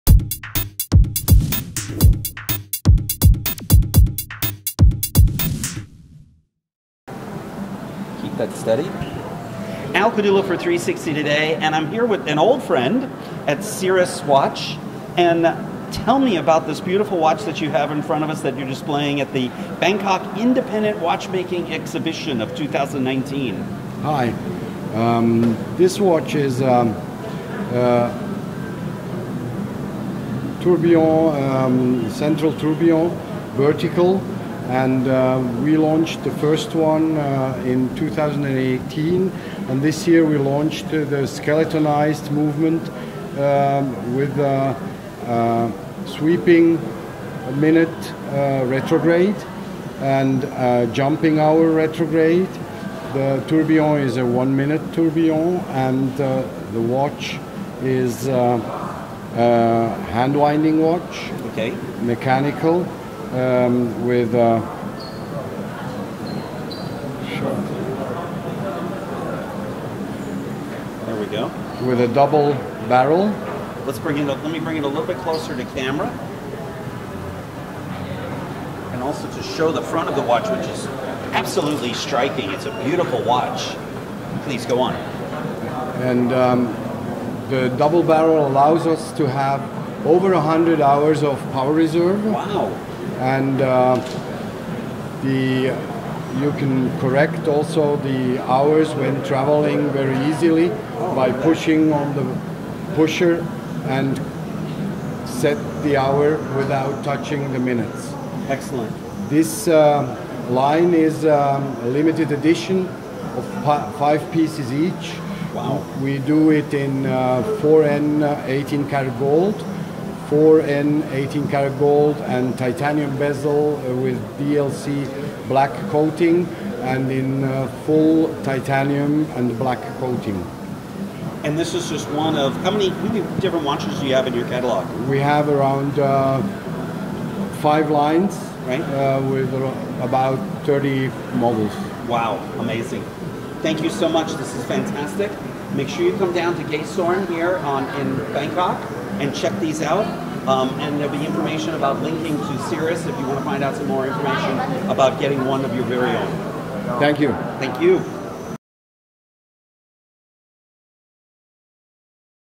Cryus Watches Proudly Shows Off In Bangkok
Cryus Watches described as a perfect balance between tradition and innovation. A three-year-old company that has taken the watch world by storm with it's unique and elegant designs. Walter Ribaga, Managing Director, proudly showed off these stunning timepieces at the first-ever Bangkok Independent Watchmakers Exhibition at Gaysorn Village in Bangkok.